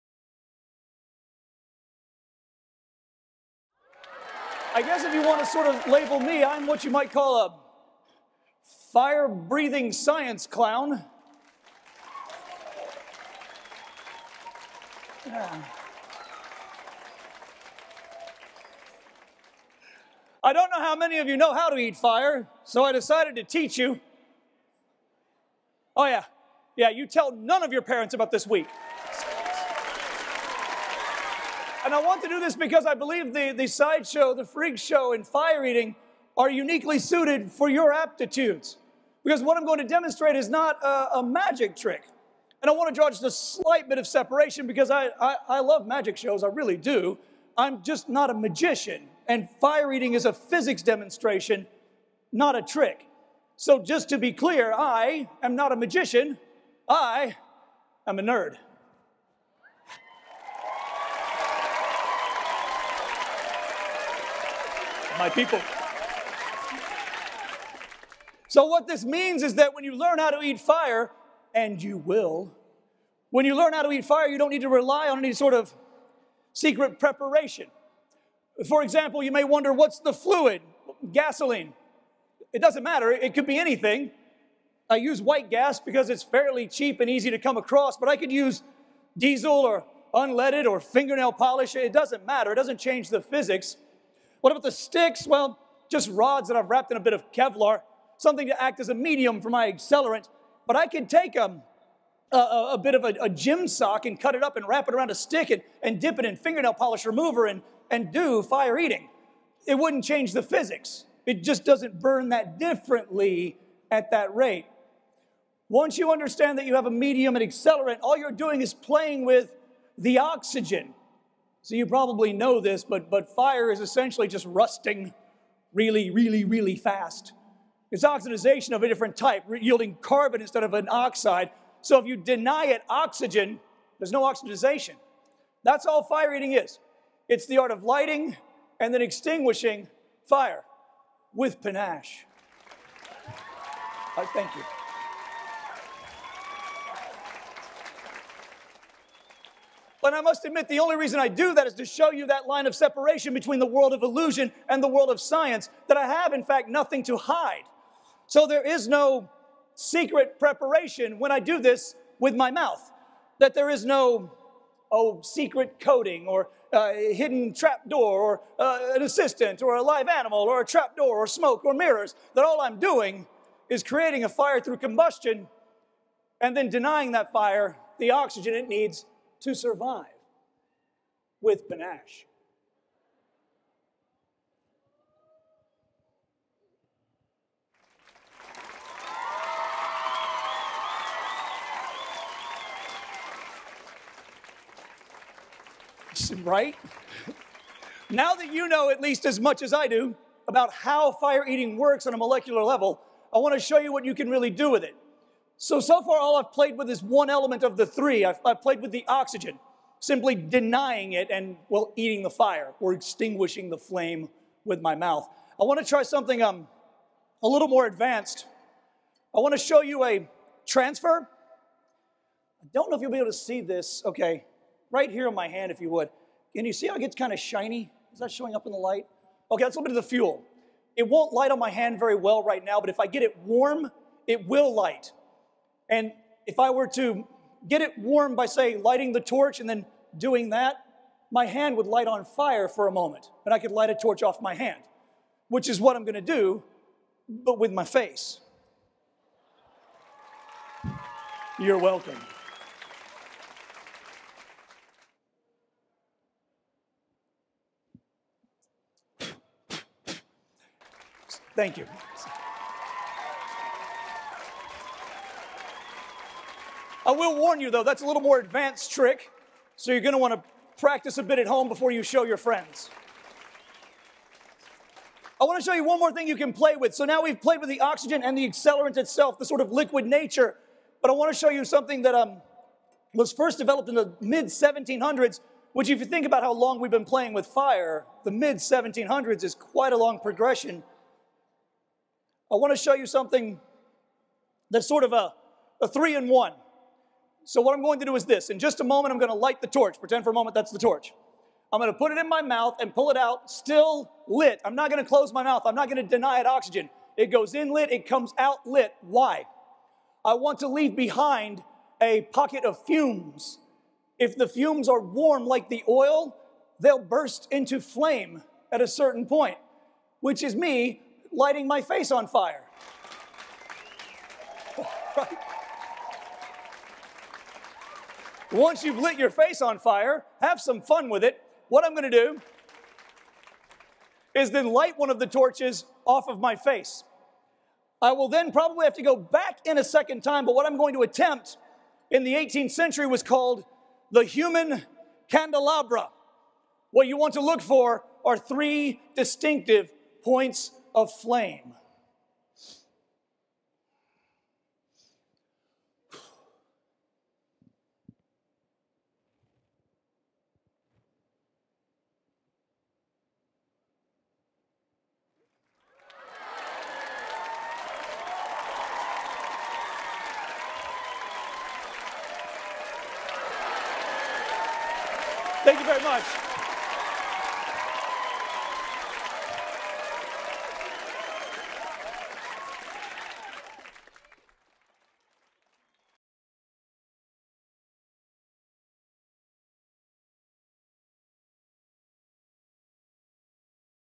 Fire Eating Is NOT Magic - It Is SCIENCE!
Thom Britton explains how fire eating works.